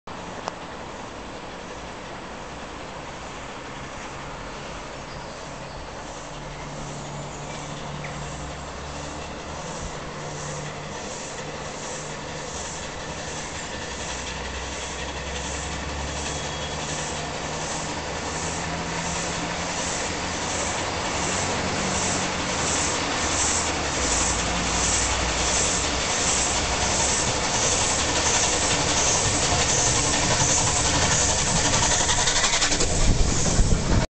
850 Lord Nelson Salisbury 23 June
Lord Nelson heading towards Salisbury Tunnel Junction